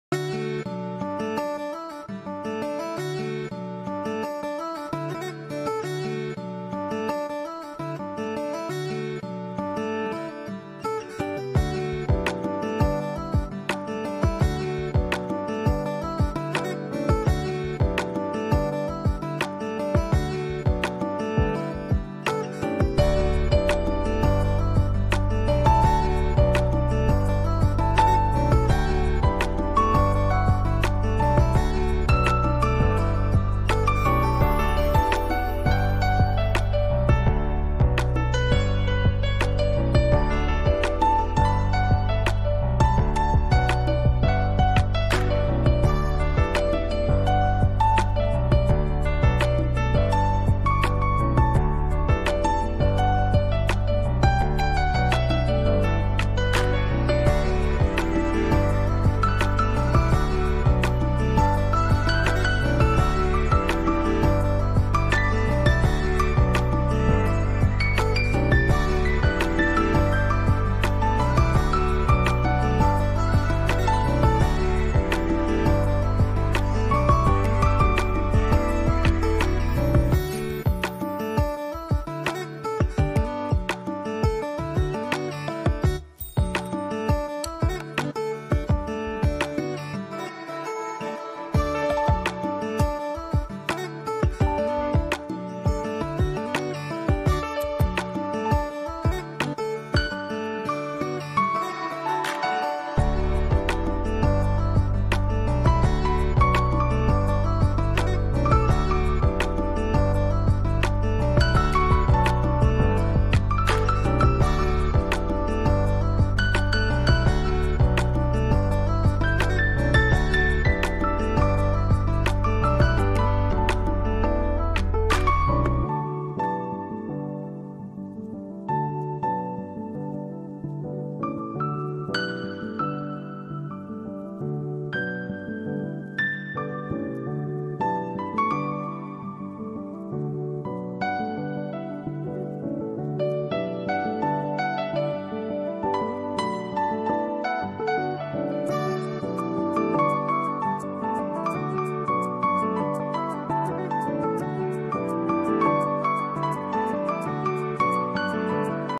SALEM-KANNAN KURICHI-2 BHK- SOUTH FACING-55 LAKHS-86085 66046
LAND-1000 SFT
BUILDING-900SFT
SOUTH FACING
LAND & BUILDING- APPROVED
FULLY INTERIOR
MICROWAVE OVEN
CHIMNEY, WATER FILTER